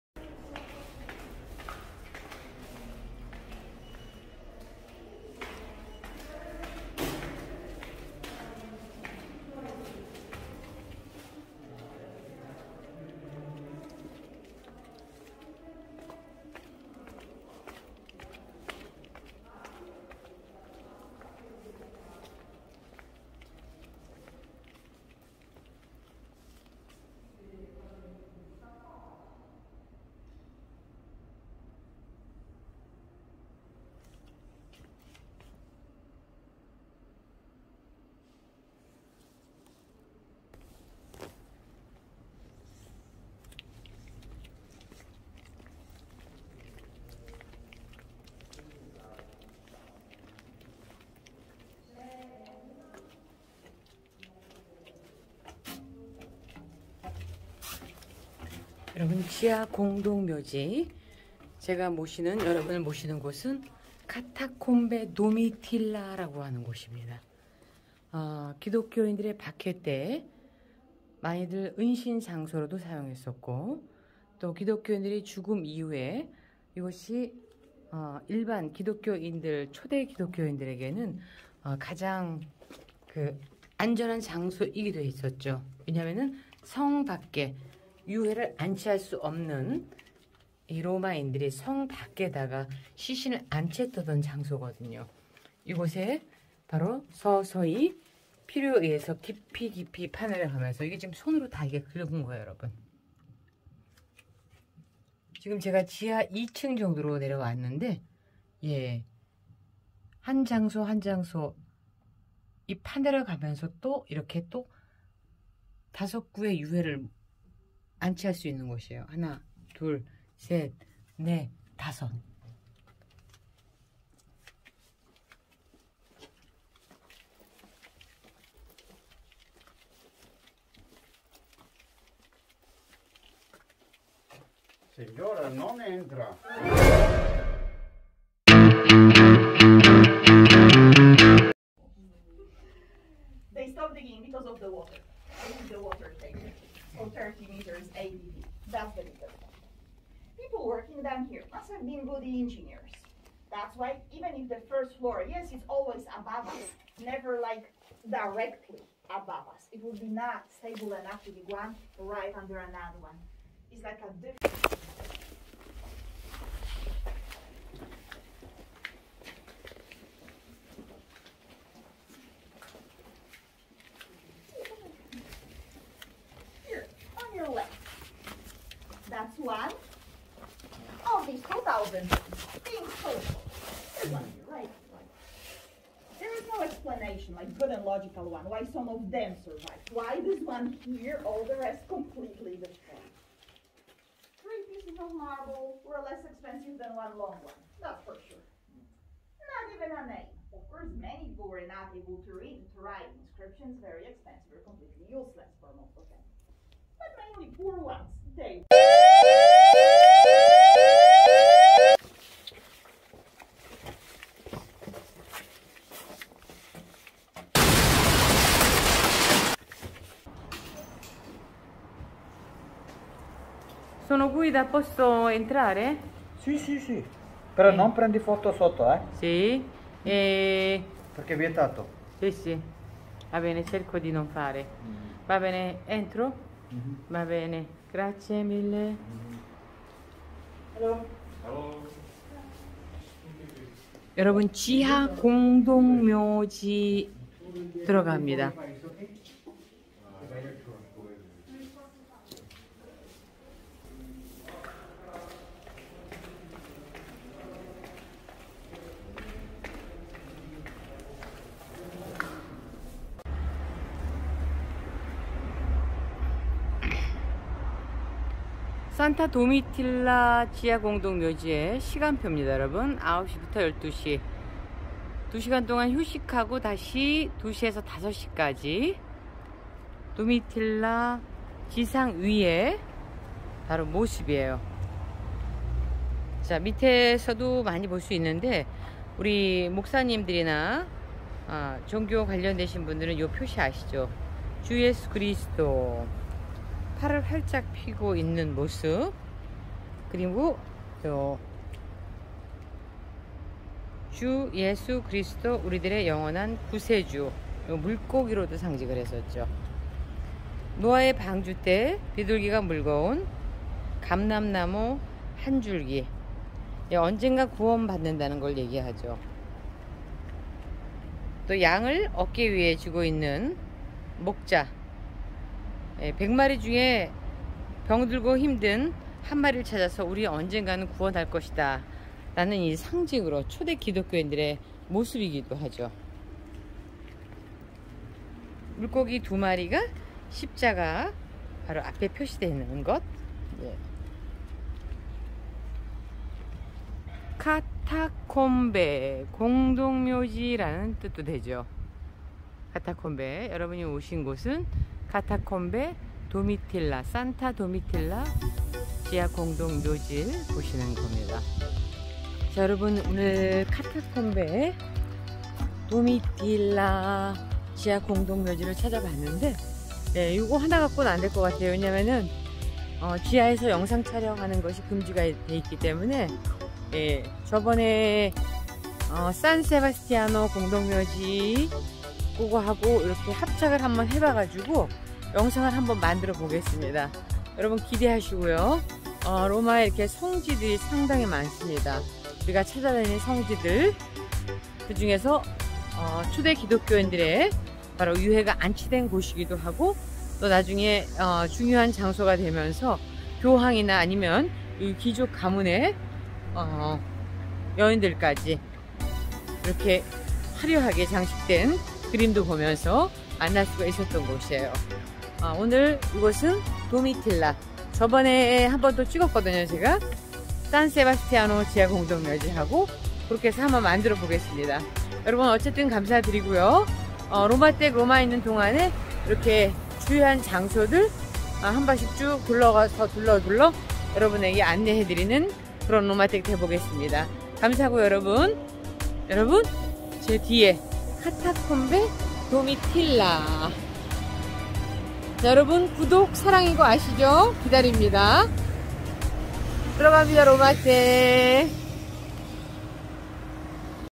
🇮🇹공동묘지에서 생긴일
9월말 로마 성지 카타콤베를 찾았어요 로마댁 욕구를 못참고 촬영하다가 추방됐어요ㅠㅠ 절대금지 절대로 촬영하면 안되는 장소!!죄송합니다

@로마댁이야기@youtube